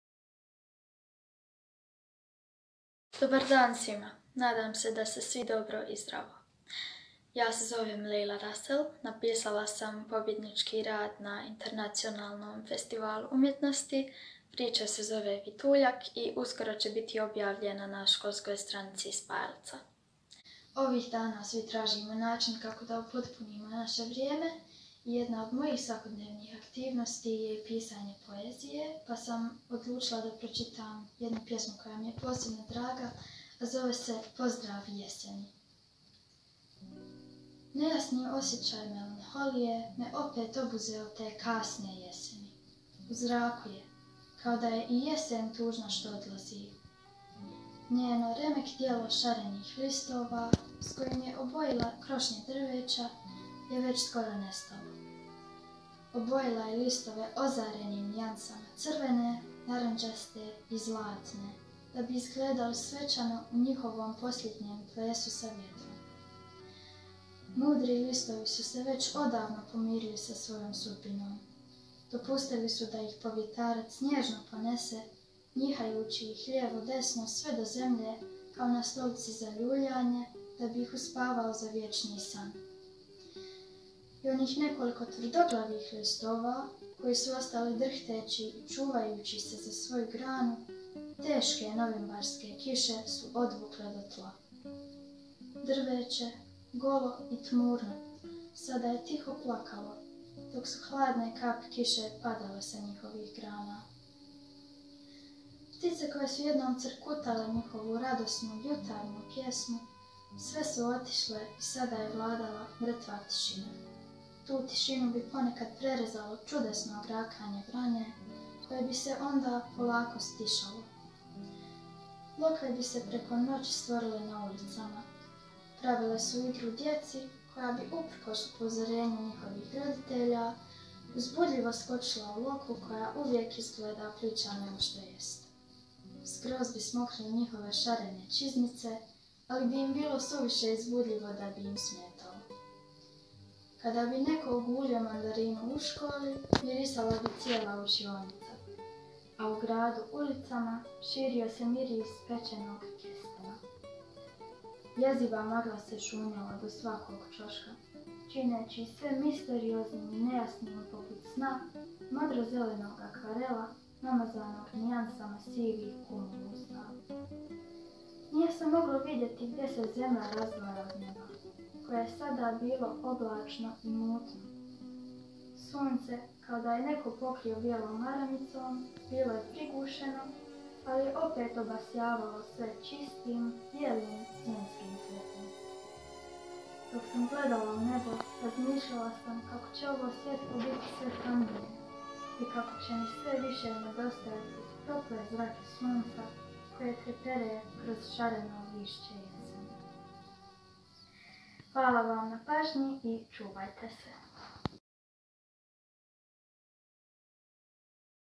Leila Russell, pobjednik FEDU 2020
Prvonagrađena na regionalnom konkursu za najljepšu neobjavljenu bajku 2020